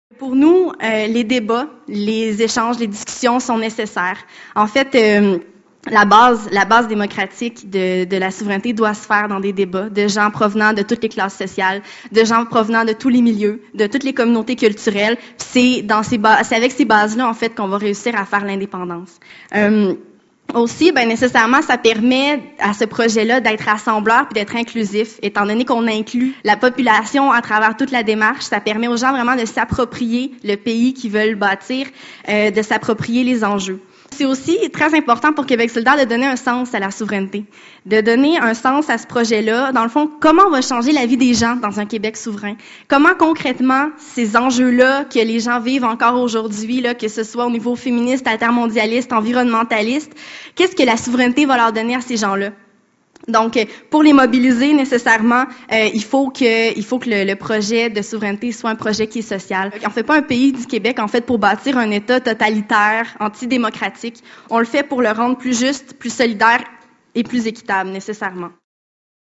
Un pays du Québec plus équitable
Montréal, 13 août 2010 - La représentante jeunesse de Québec solidaire, Émilie Guimond-Bélanger, a profité de son discours d'ouverture dans le cadre de l'Université d'été pour expliquer à l'auditoire sa vision d'un pays du Québec plus juste, plus solidaire et plus équitable.